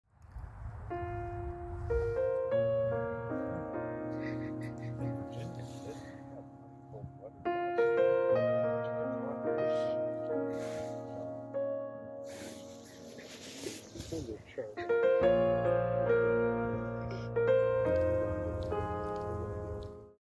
Watch for the Love of Dog and Little Boy (一）
Cairn Terrier dog is very friendly with kids, Neighbor and little boy come to visit,  Dog Benji is excited and plays with him.  Most loving dog in the world.